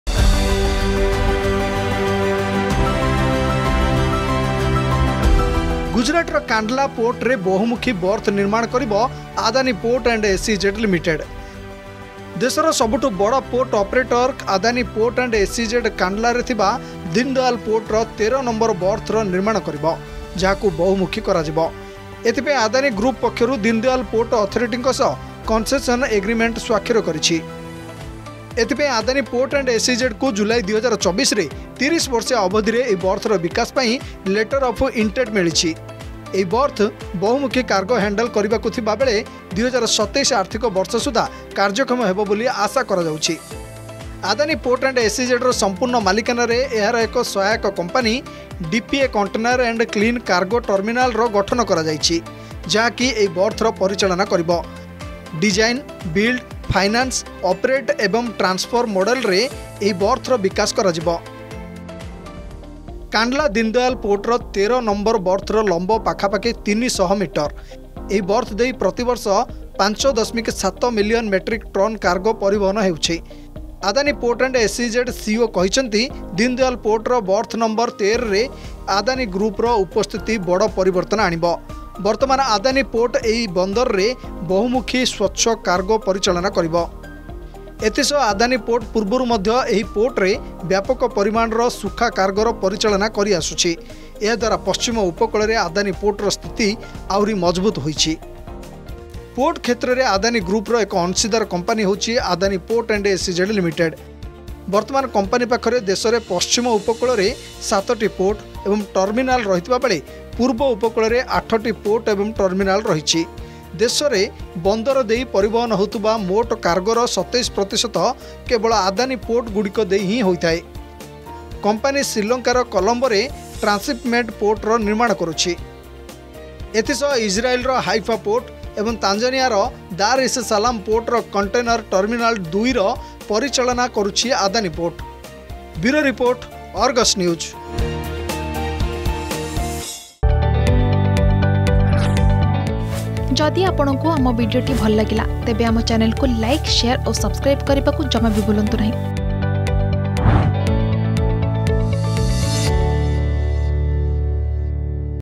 ଗୁଜରାଟର କାଣ୍ଡଲା ପୋର୍ଟରେ ବହୁମୁଖୀ ବର୍ଥ ନିର୍ମାଣ କରିବ ଆଦାନୀ ପୋର୍ଟ ଆଣ୍ଡ ଏସଇଜେଡ୍ ଲିମିଟେଡ୍ || Adani Group
ଗୁଜରାଟର କାଣ୍ଡଲା ପୋର୍ଟରେ ବହୁମୁଖୀ ବର୍ଥ ନିର୍ମାଣ କରିବ ଆଦାନୀ ପୋର୍ଟ ଆଣ୍ଡ ଏସଇଜେଡ୍ ଲିମିଟେଡ୍ ।  ଦେଶର ସବୁଠୁ ବଡ ପୋର୍ଟ ଅପେରଟର ଆଦାନୀ ପୋର୍ଟ ଆଣ୍ଡ ଏସଇଜେଡ୍  କାଣ୍ଡଲାରେ ଥିବା ଦିନଦୟାଲ ପୋର୍ଟର ୧୩ ନମ୍ବର ବର୍ଥର ନିର୍ମାଣ କରିବ।